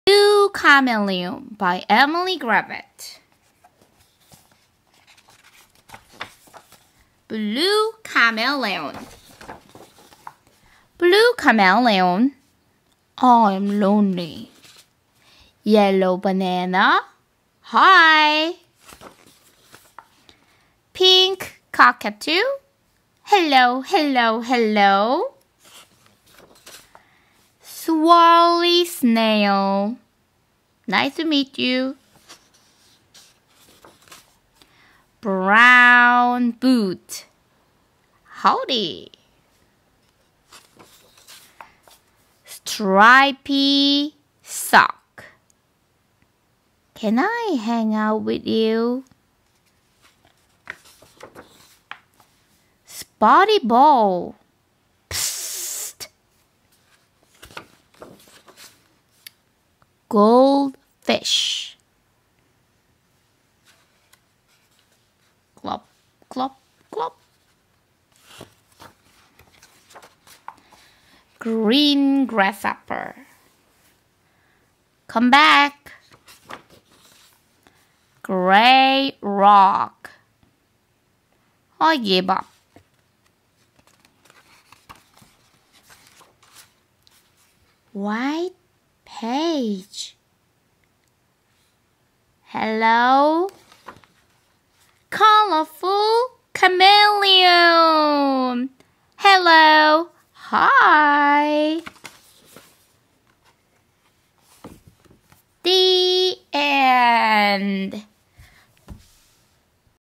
Blue Chameleon / children's book reading
다양한 변신의 귀재 카멜레온의 이야기